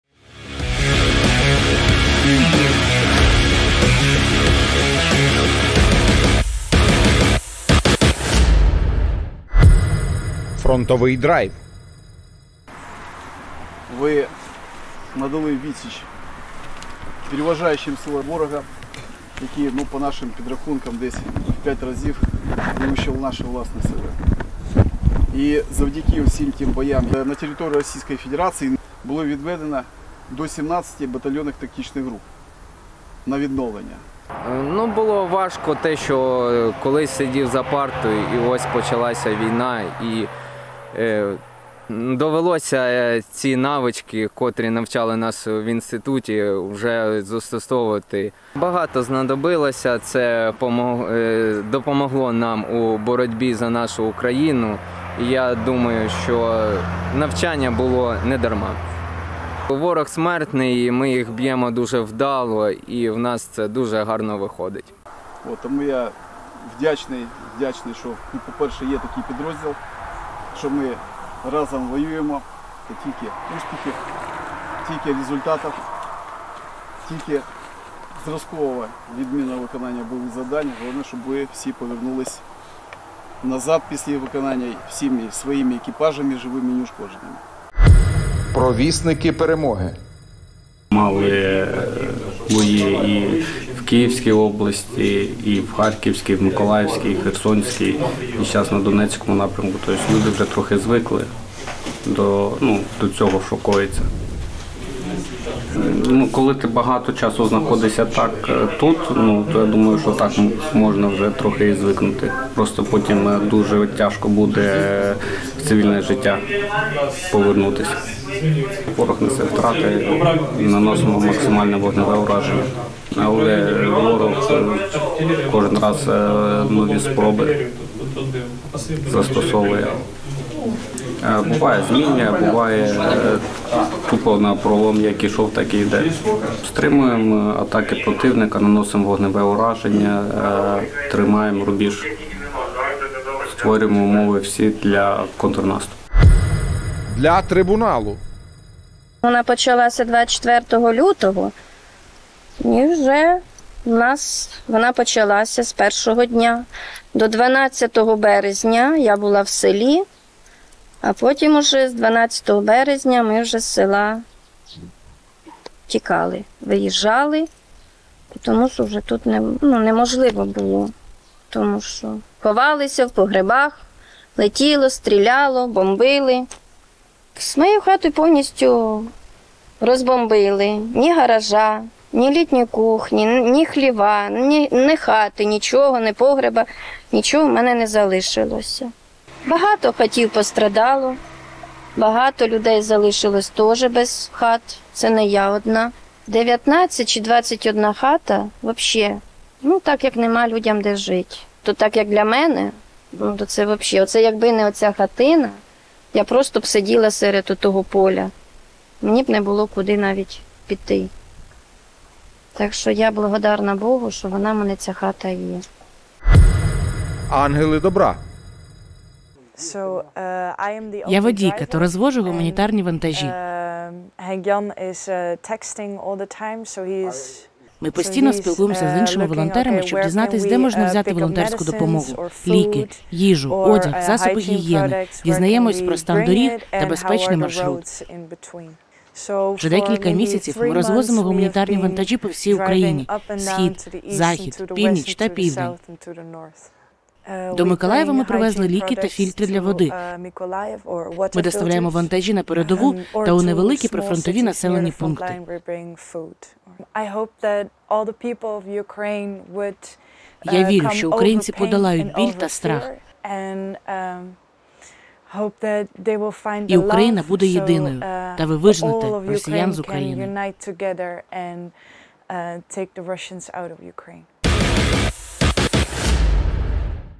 Випуск ФОРПОСТ.UA від 03.08.2022
У цьому випуску:
0.00 Фронтовий драйв - Командувач Сухопутні війська ЗС України
1.30 Провісники перемоги – наші захисники
2.28 Для Гааги – злочини росіян на Київщині
3.38 Ангели добра - волонтери з Нідерландів
Джерело: телеграм-канал ФОРПОСТ.UA